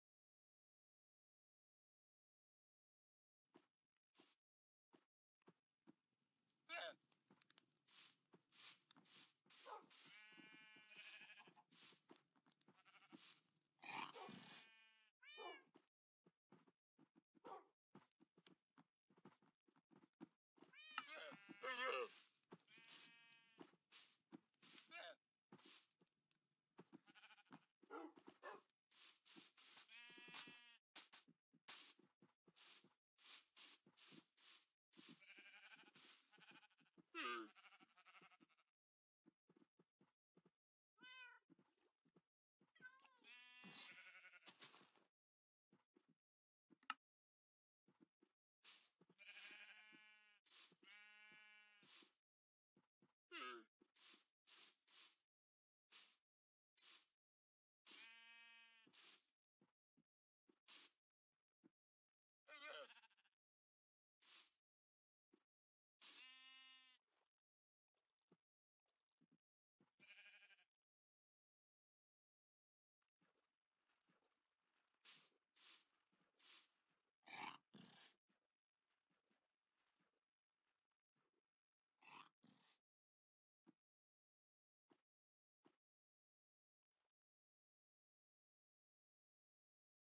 Two Wandering Traders in my World!!!
It is a surprising experience seeing two Wandering Traders naturally spawn in my world. Enjoy the video.